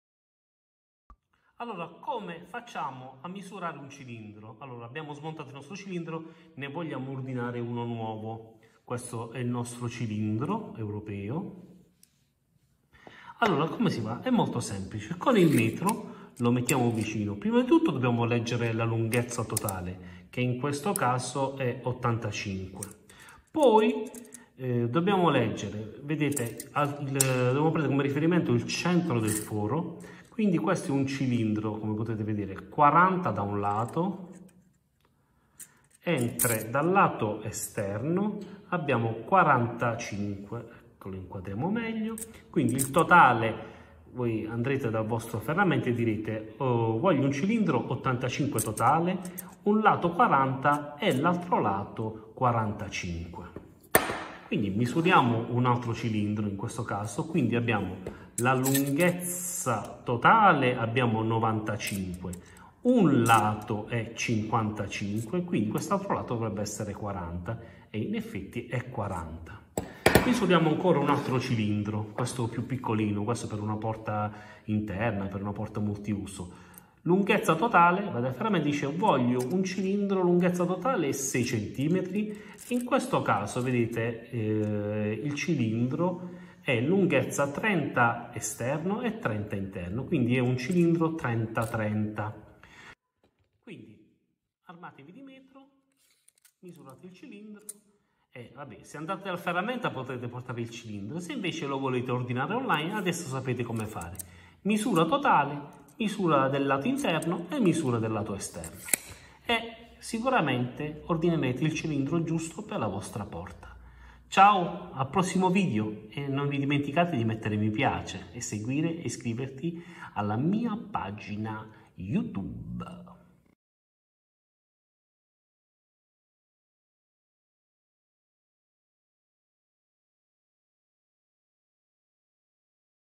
Come misurare un cilindro
In questo video ti ho mostrato come smontare un cilindro dalla serratura di una porta blindata
In questo video ti spiego in maniera semplice come prendere la misura per ordinare uno nuovo cilindro, senza sbagliare.
Se hai bisogno di fare delle domande, scrivile pure nei commenti , rispondo sempre.